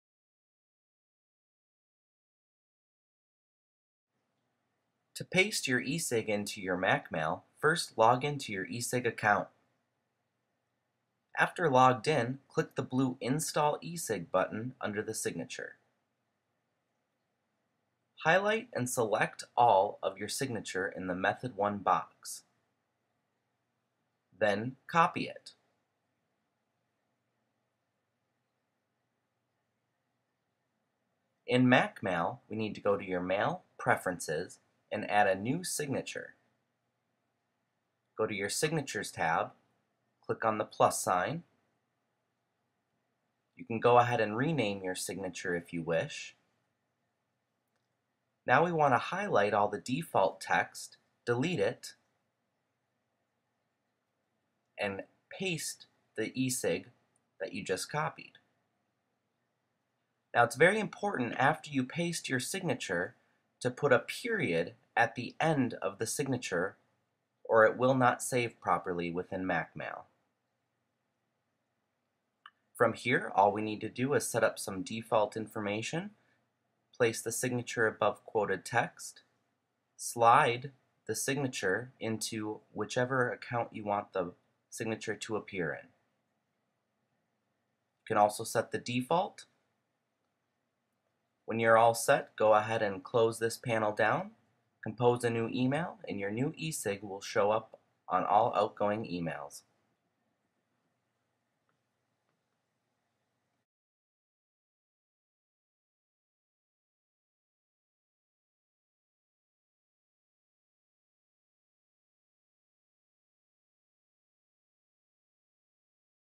Mac Mail HTML Email Signature Instructions
How to copy and paste your email signature into MacMail email.  HTML email signature installation instructions for mac mail.